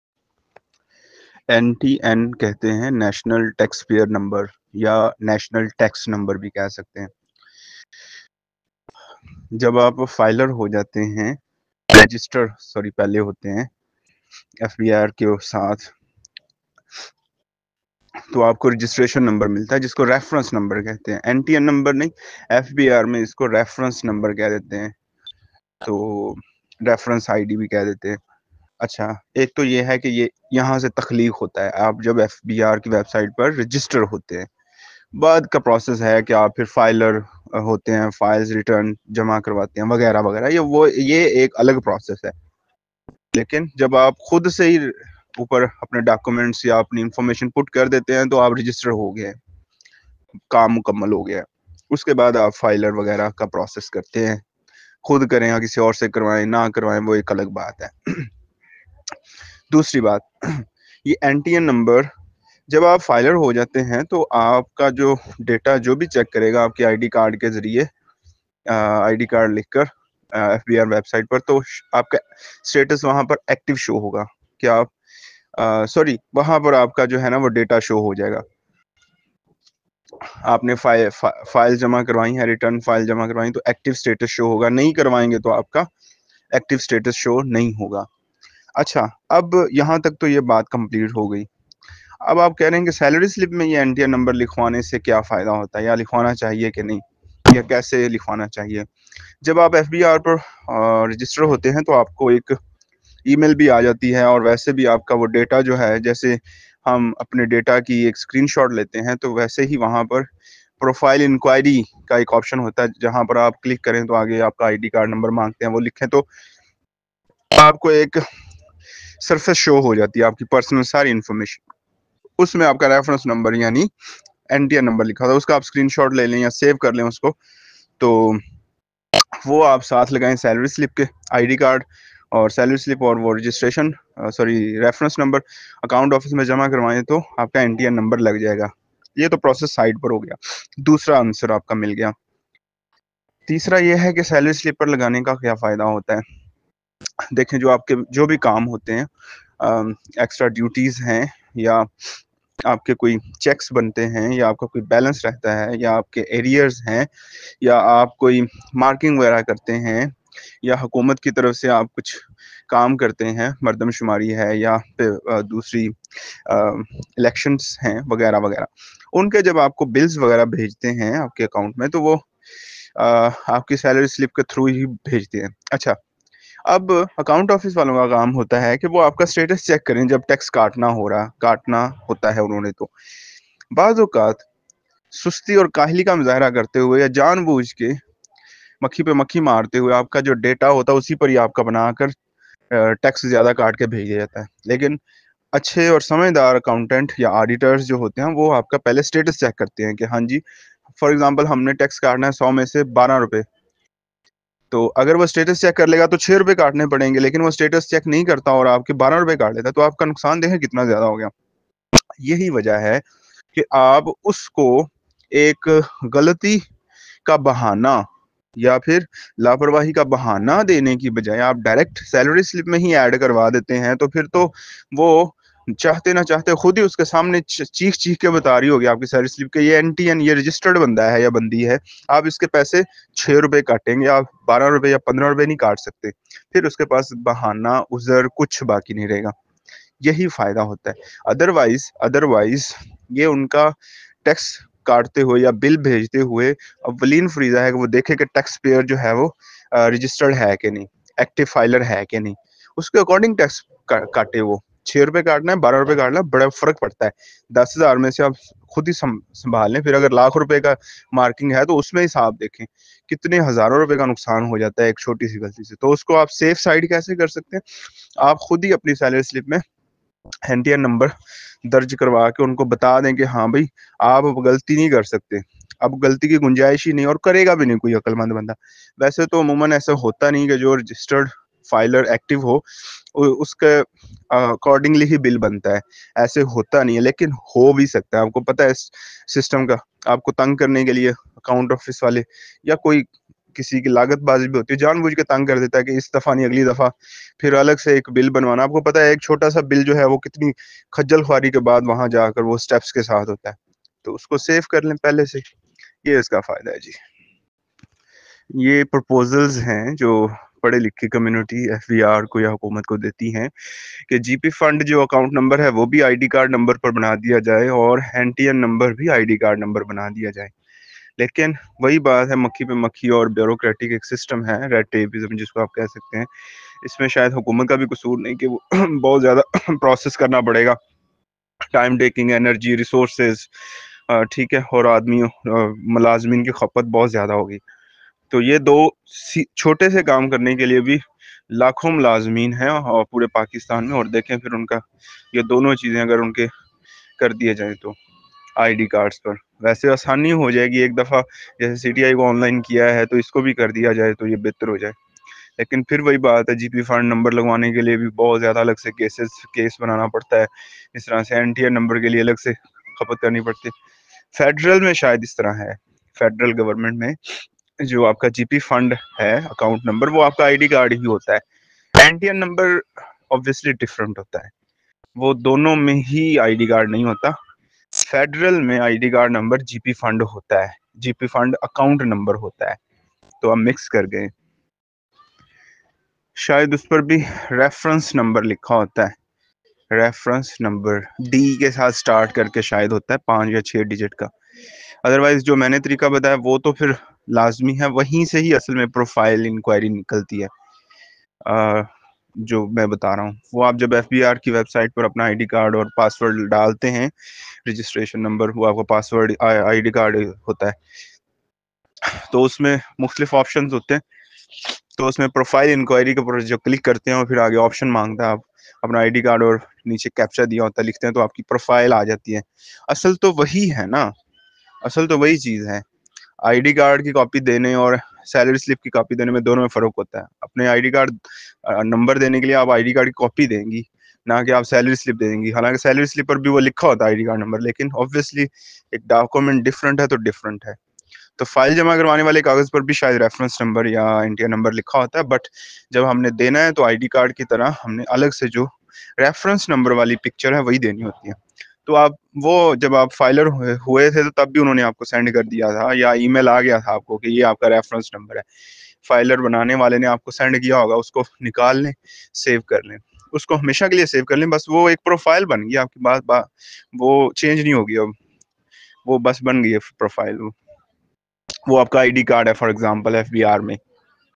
NTN meaning? How to get NTN number? ID card is NTN? Entering NTN in Salary Slip
NTN meaning?
How to get NTN?
ID_card is NTN?
Entering NTN in Salary_Slip